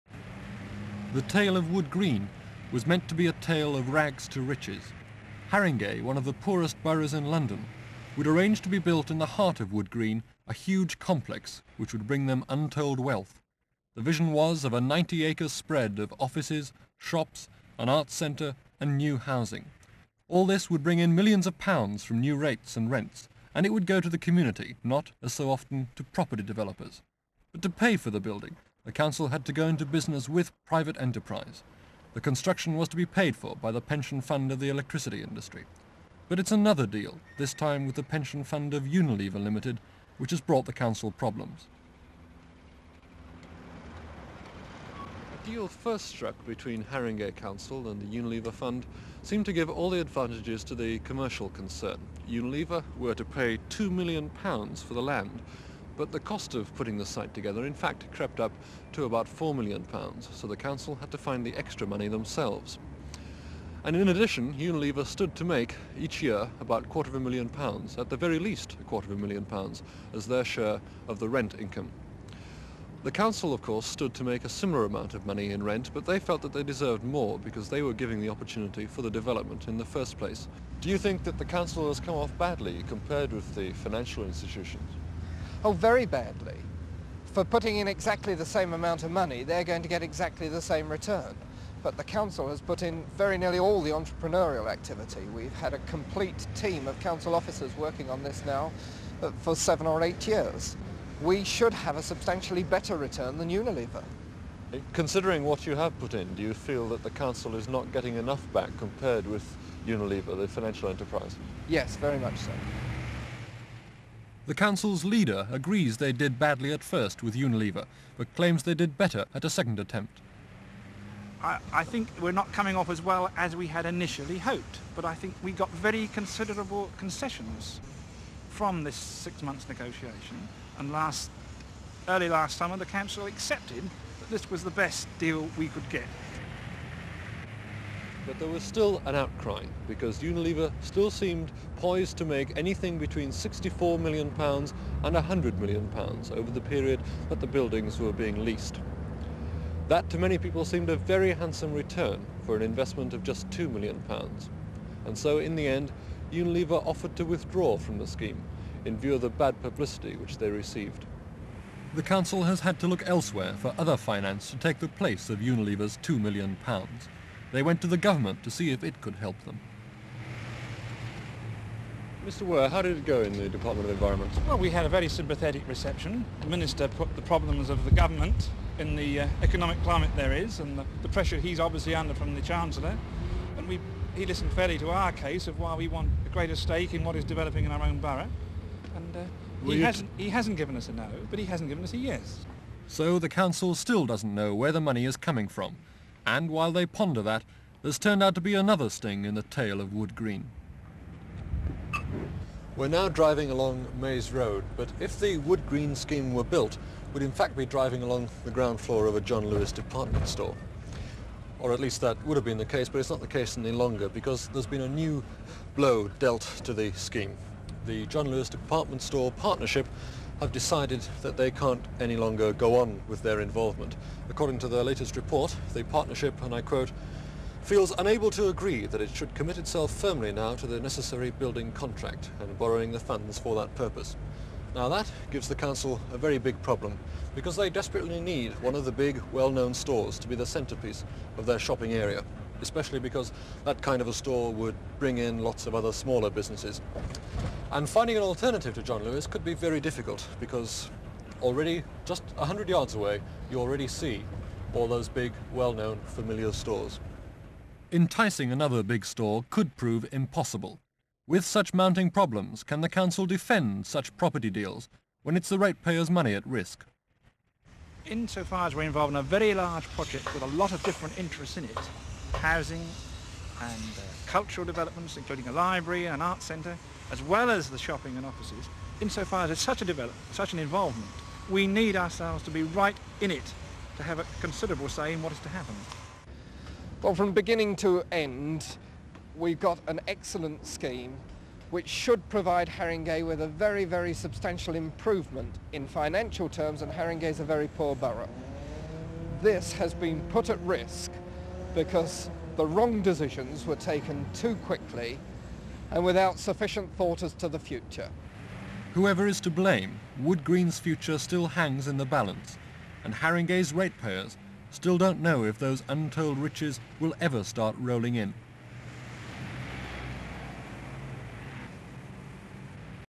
Wood Green (1974-1976)
Unissued / unused material - dates and locations unclear or unknown.

Colour item.

Wood Green, London.

Panning shot boarded up houses.  Various model of proposed shopping centre.  Various shots building site.  CU news reporter talking to camera about conflict between Unilever and Haringey Borough Council over building of shopping centre.  Various shots representatives from Haringey council.  CU page from Hornsey Journal newspaper. CU reporter.  Various shots council representatives visiting Department of Environment. Travel shot of street in Wood Green taken from moving car, in foreground reporter sits in passenger seat and talks to camera. Shops include famous chain stores such as Marks and Spencers, Woolworths and Boots.  More shots shopping centre model.  More shots council representative speaking to camera.  LS Wood Green, zoom out from Alexandra Palace.
 FILM ID:3411.08